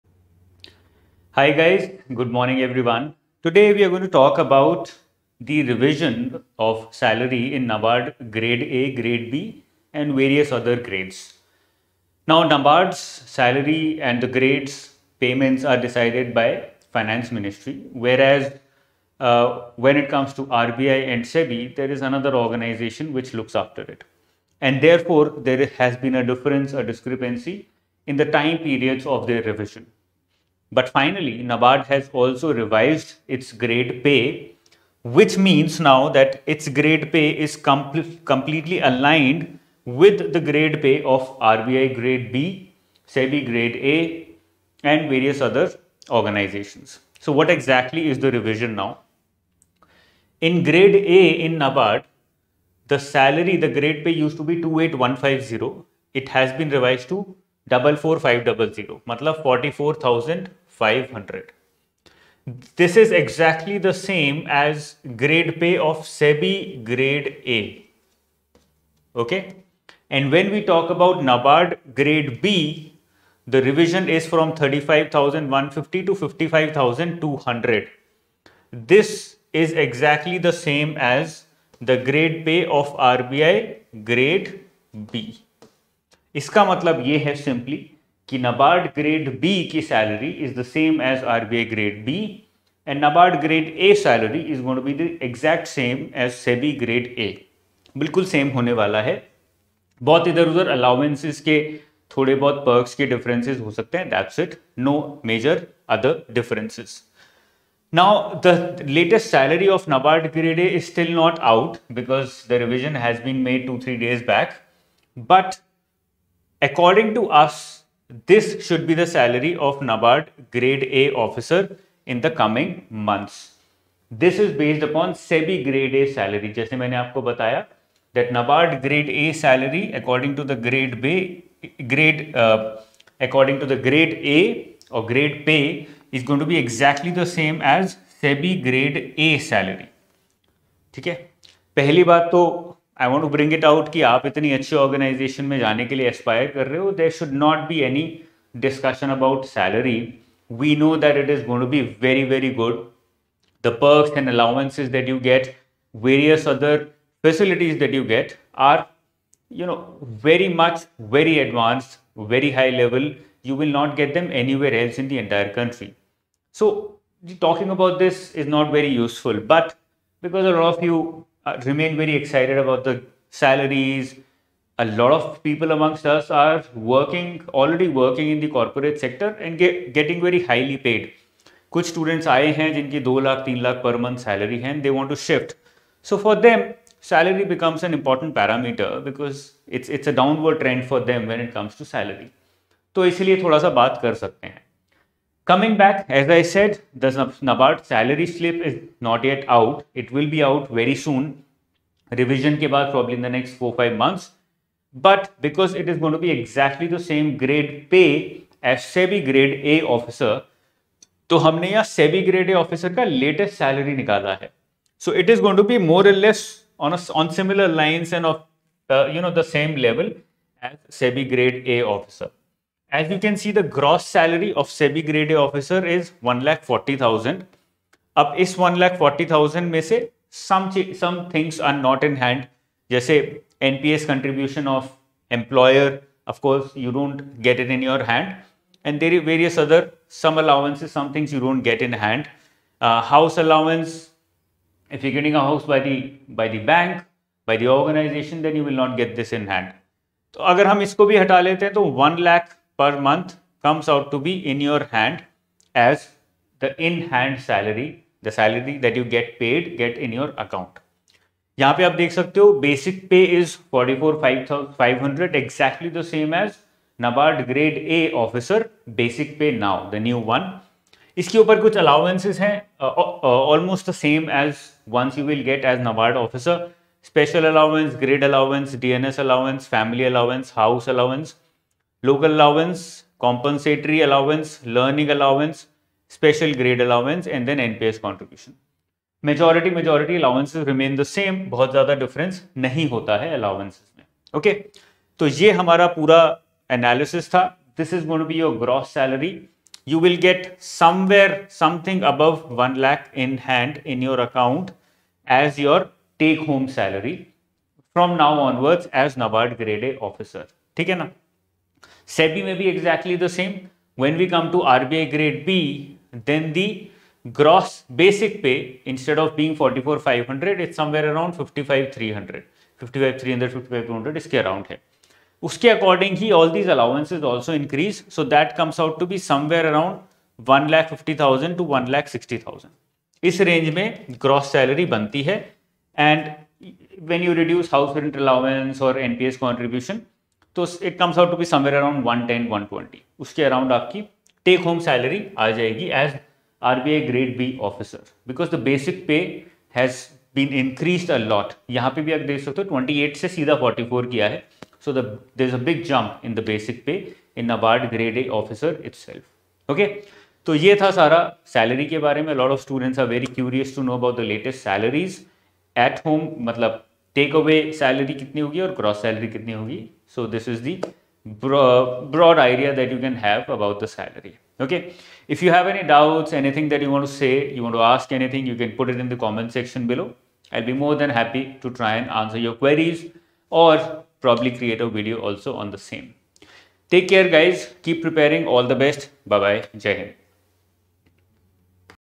Revised Salary of NABARD | NABARD Latest Salary 2022 | NABARD Grade A and B New Salary
A recent notice released by NABARD has intimated changes in the salaries of all the officer grade posts at NABARD. What is the change? How much increase has been induced? What are the new figures? And of course, What is the new salary of NABARD Grade A officer? To get answers of all these questions, watch this session.